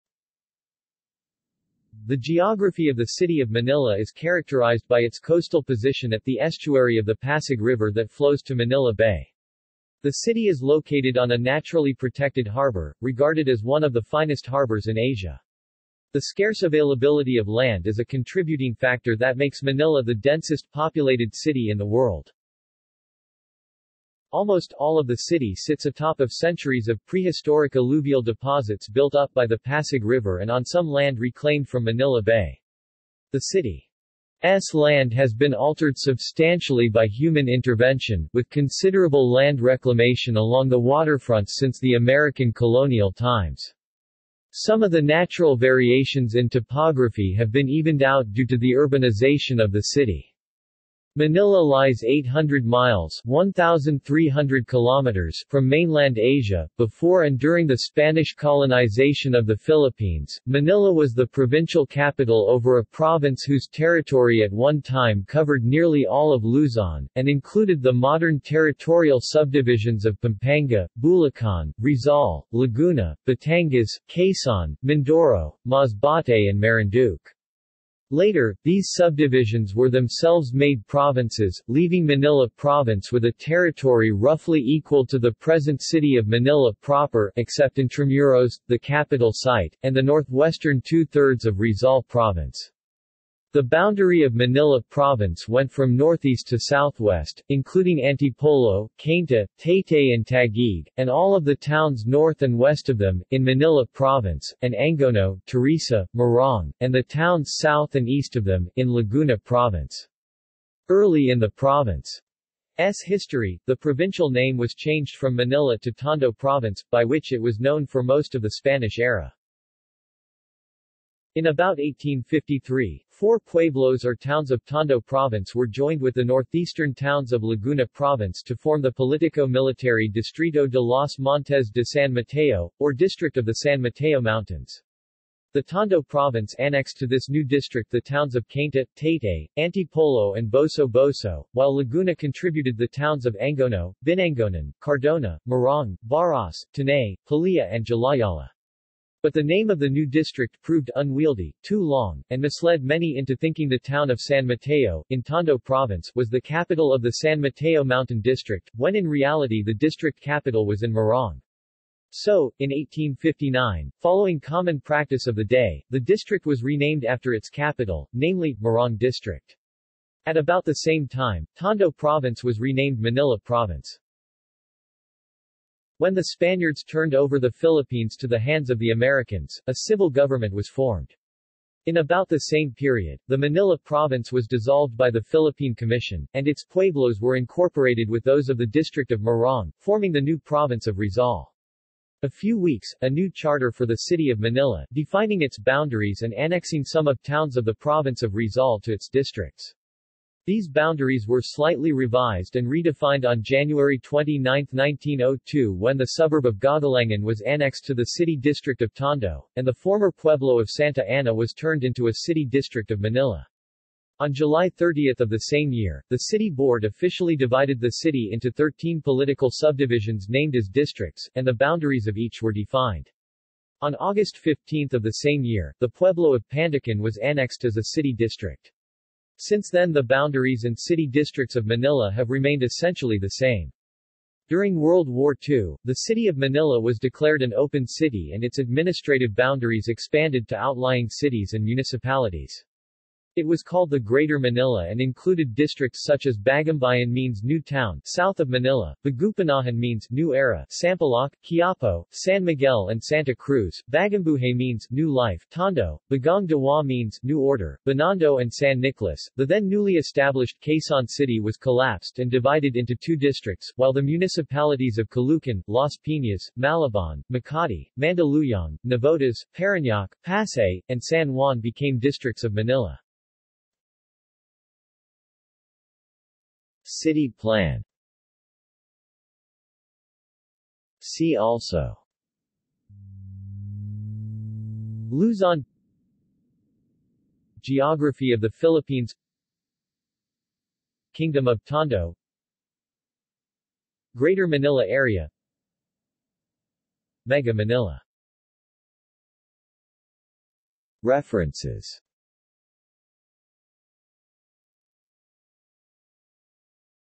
Geography of Manila | Wikipedia audio article
This is an audio version of the Wikipedia Article:
Geography of Manila

SUMMARY
The geography of the City of Manila is characterized by its coastal position at the estuary of the Pasig River that flows to Manila Bay. The city is located on a naturally protected harbor, regarded as one of the finest harbors in Asia. The scarce availability of land is a contributing factor that makes Manila the densest populated city in the world.
Almost all of the city sits atop of centuries of prehistoric alluvial deposits built up by the Pasig River and on some land reclaimed from Manila Bay. The city's land has been altered substantially by human intervention, with considerable land reclamation along the waterfronts since the American colonial times. Some of the natural variations in topography have been evened out due to the urbanization of the city. Manila lies 800 miles (1,300 km) from mainland Asia.Before and during the Spanish colonization of the Philippines, Manila was the provincial capital over a province whose territory at one time covered nearly all of Luzon, and included the modern territorial subdivisions of Pampanga, Bulacan, Rizal, Laguna, Batangas, Quezon, Mindoro, Masbate and Marinduque. Later, these subdivisions were themselves made provinces, leaving Manila province with a territory roughly equal to the present City of Manila proper (except Intramuros, the capital site), and the northwestern two-thirds of Rizal province. The boundary of Manila province went from northeast to southwest, including Antipolo, Cainta, Taytay and Taguig, and all of the towns north and west of them, in Manila province; and Angono, Teresa, Morong, and the towns south and east of them, in Laguna province. Early in the province's history, the provincial name was changed from Manila to Tondo Province, by which it was known for most of the Spanish era.
In about 1853, four pueblos or towns of Tondo Province were joined with the northeastern towns of Laguna province to form the politico-military Distrito de los Montes de San Mateo, or District of the San Mateo Mountains. The Tondo Province annexed to this new district the towns of Cainta, Taytay, Antipolo and Boso-boso, while Laguna contributed the towns of Angono, Binangonan, Cardona, Morong, Baras, Tanay, Pililla and Jalajala. But the name of the new district proved unwieldy, too long, and misled many into thinking the town of San Mateo (in Tondo province) was the capital of the San Mateo Mountain District, when in reality the district capital was in Morong. So, in 1859, following common practice of the day, the district was renamed after its capital; namely, Morong District. At about the same time, Tondo Province was renamed Manila Province.
When the Spaniards turned over the Philippines to the hands of the Americans, a civil government was formed. In about the same period, the Manila Province was dissolved by the Philippine Commission, and its pueblos were incorporated with those of the District of Morong, forming the new Province of Rizal. A few weeks, a new charter for the City of Manila, defining its boundaries and annexing some of towns of the Province of Rizal to its districts. These boundaries were slightly revised and redefined on January 29, 1902 when the suburb of Gagalangin was annexed to the city district of Tondo, and the former pueblo of Santa Ana was turned into a city district of Manila. On July 30 of the same year, the city board officially divided the city into 13 political subdivisions named as districts, and the boundaries of each were defined. On August 15 of the same year, the pueblo of Pandacan was annexed as a city district. Since then the boundaries and city districts of Manila have remained essentially the same.
During World War II, the city of Manila was declared an open city and its administrative boundaries expanded to outlying cities and municipalities. It was called the Greater Manila and included districts such as Bagumbayan means New Town (South of Manila), Bagumpanahon means "New Era" (Sampaloc, Quiapo, San Miguel and  ...